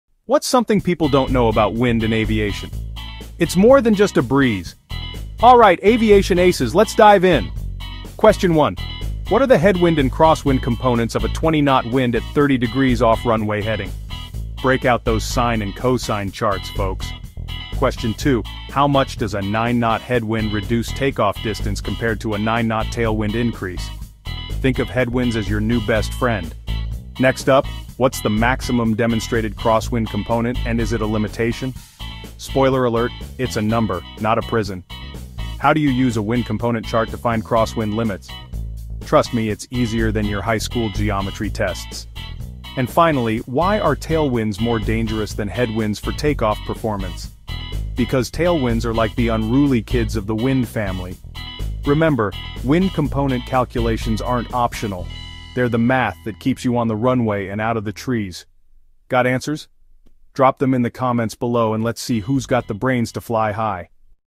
How Crosswinds Make Landings Wild Quiz 03!
Question Series 3/5
Question Series 3: "Wind Component Mastery"
Questions:
"What are the headwind and crosswind components of a 20-knot wind at 30 degrees off runway heading?"
"How much does a 9-knot headwind reduce takeoff distance compared to a 9-knot tailwind increase?"
"What's the maximum demonstrated crosswind component, and is it a limitation?"
"How do you use a wind component chart to find crosswind limits?"
"Why are tailwinds more dangerous than headwinds for takeoff performance?"